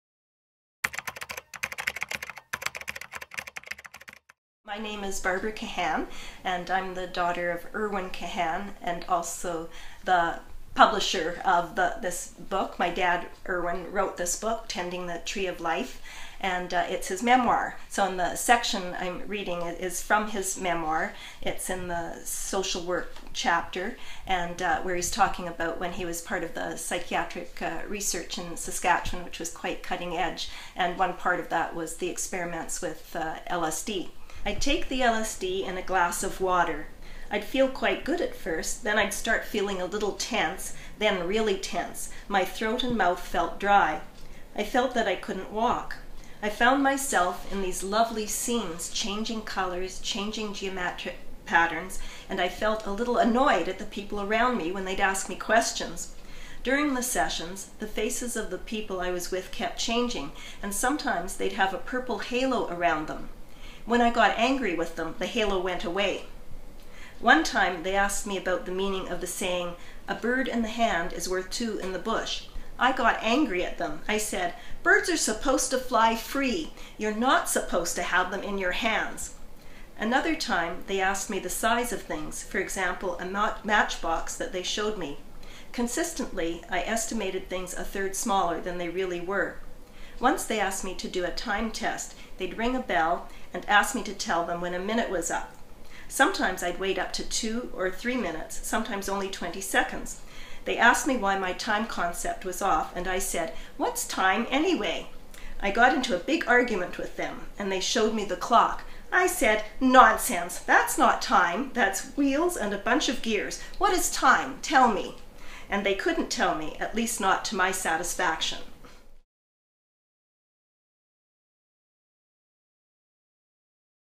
Tending the Tree of Life, a memoir
Barbara Kahan speaks about her father Irwin and Tending the Tree of Life, a memoir being released Monday.